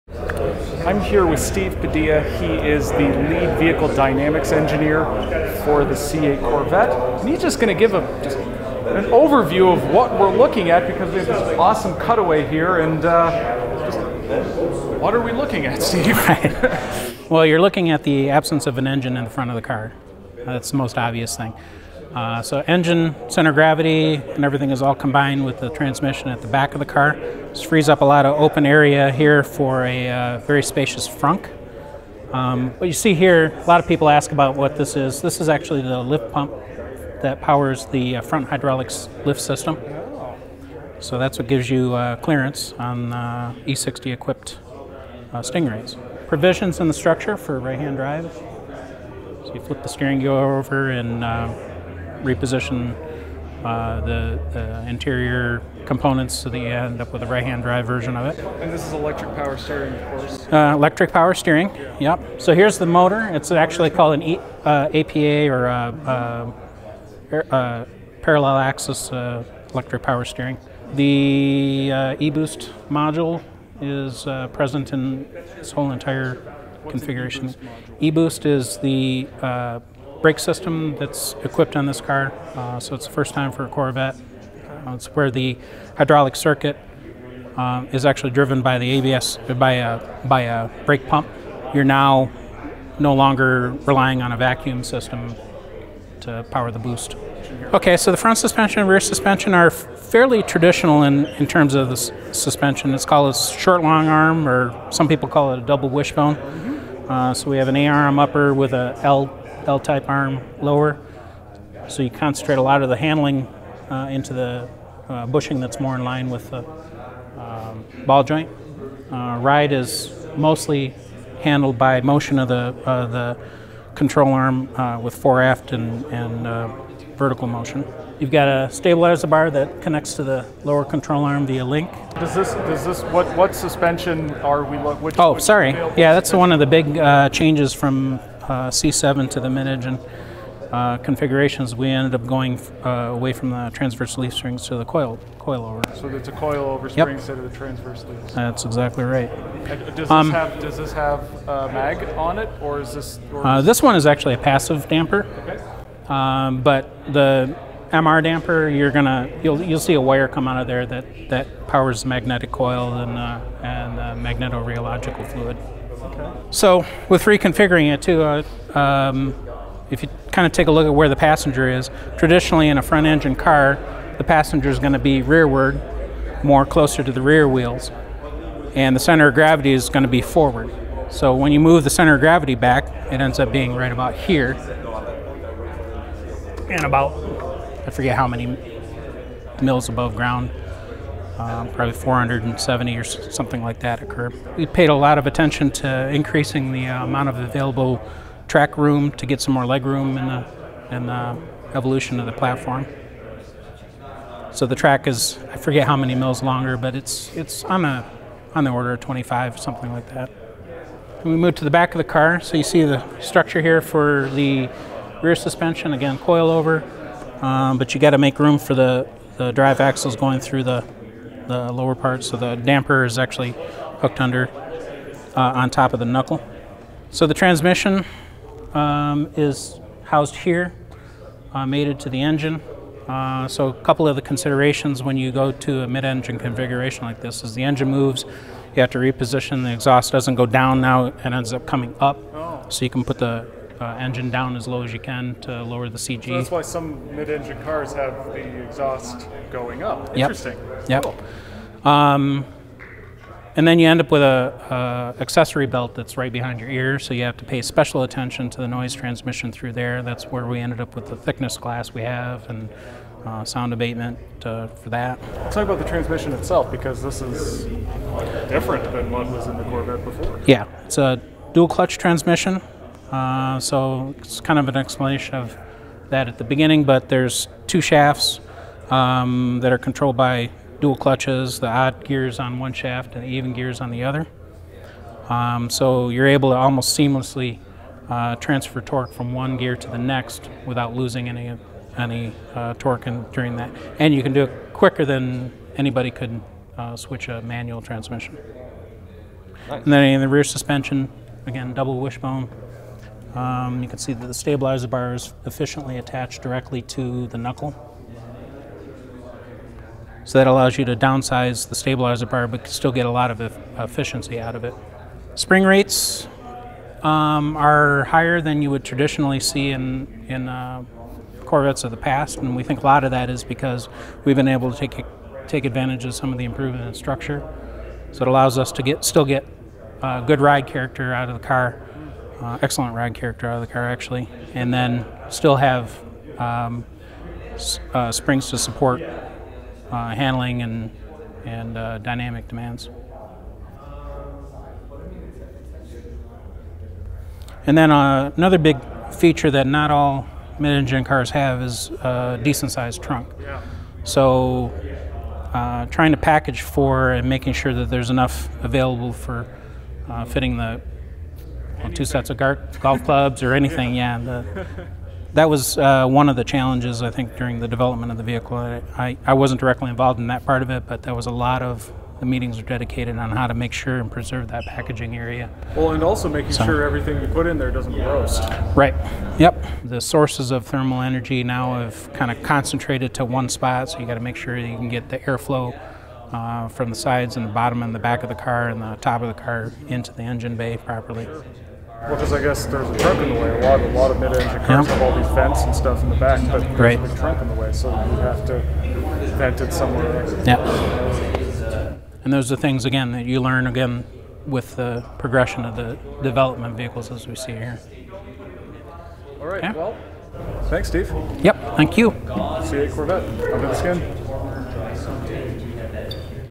Take a look at the guts of the 2020 Chevrolet Corvette Stingray
Ever wonder what’s under the exterior Chevy’s new mid-engine sports car? Take a look inside the all-new 2020 Chevrolet Corvette Stingray.